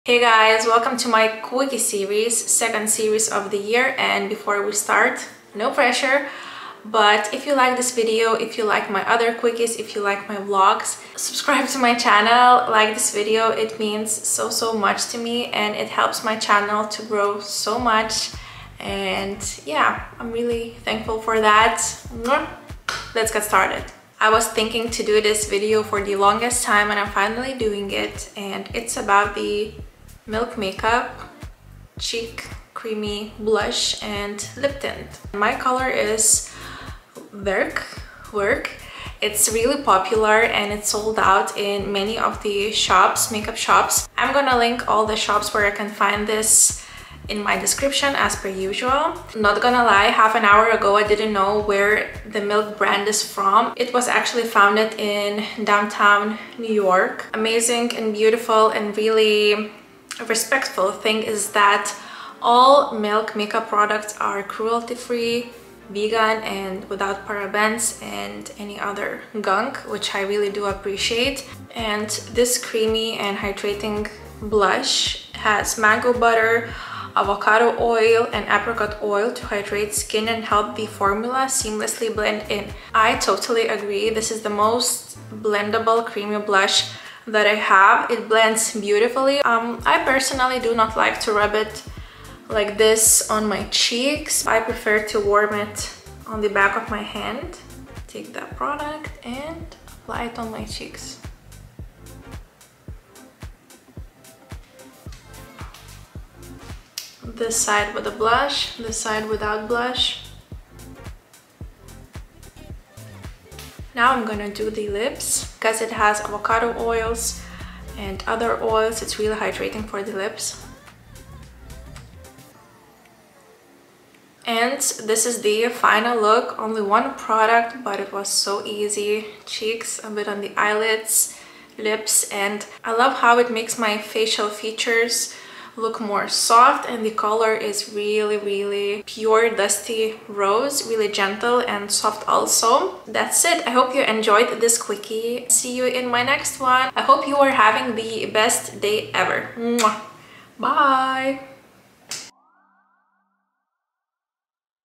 QUICKIE | MILK Makeup Lip + Cheek Cream Blush Stick in WERK review 2023
Welcome to my QUICKIE dedicated to MILK Makeup Lip + Cheek Cream Blush Stick in WERK. In my opinion, it is one of the best cream blushes I've ever tried and owned!

About the product:

A two-in-one cream blush and lip tint formulated with hydrating ingredients for buildable, blendable color.

This product is vegan, clean, cruelty-free, and paraben-free.

With mango butter, avocado oil, and apricot oil hydrate skin and help the formula seamlessly blend in.

MILK Makeup lip + cheek cream blush stick is available in 8 shades.